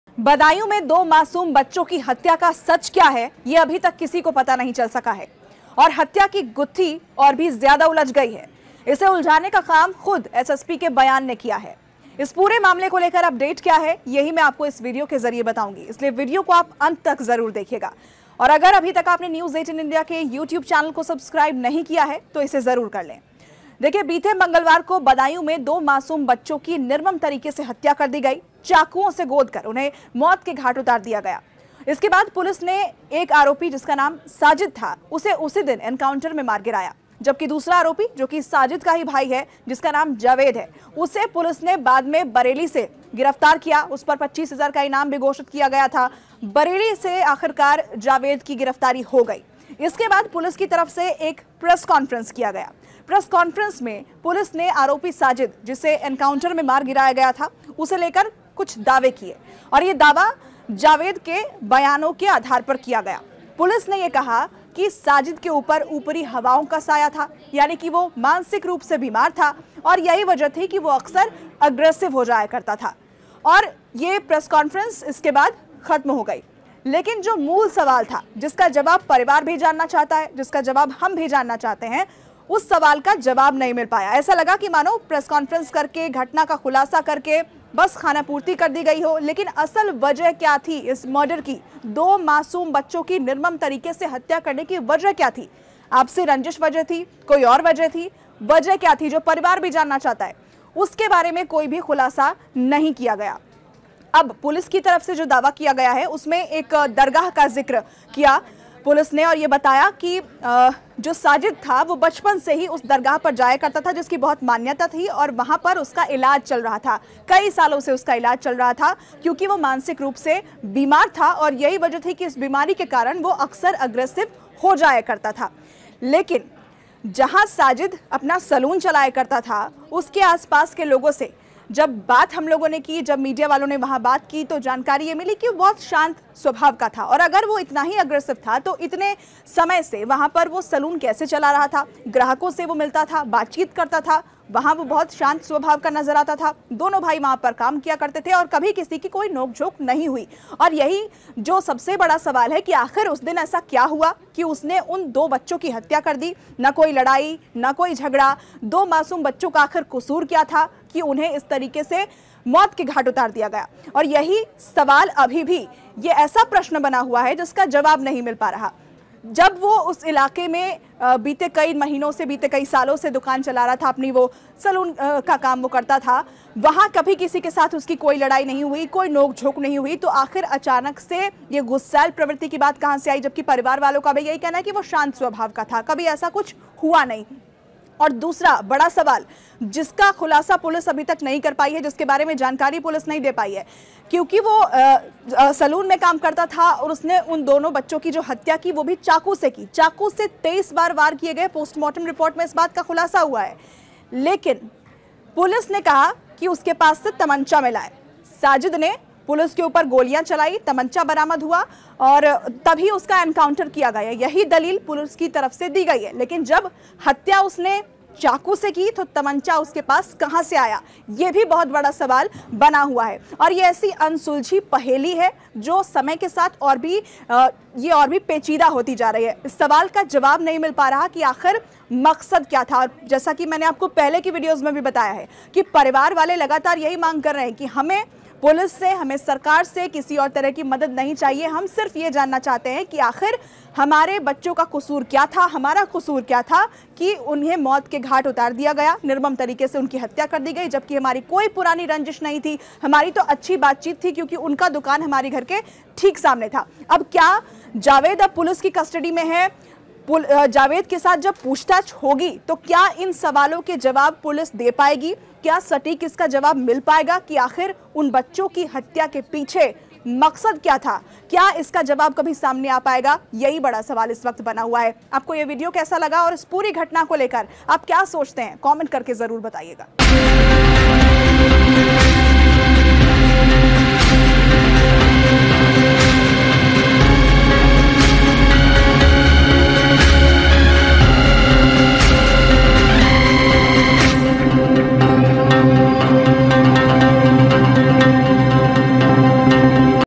Budaun Double Murder Case : बदायूं हत्याकांड में आया नया एंगल, सब हैरान ? | Latest News | News18
उत्तर प्रदेश के बदायूं में  दो बच्चों की हत्या के बाद साजिद का पुलिस ने एनकाउंटर कर दिया. वहीं 25000 का इनामी साजिद का भाई जावेद ने बरेली में सरेंडर कर दिया. इसके बाद पुलिस उसे गिरफ्तार करके बदायूं ले आई. इसके बाद आनन-फानन में एसएसपी द्वारा 6:00 बजे प्रेस कॉन्फ्रेंस की जाती है जिसमें साजिद को मानसिक बीमार बताया जाता है. ऐसा मानसिक बीमार जो अक्सर एग्रेसिव भी हो जाता था.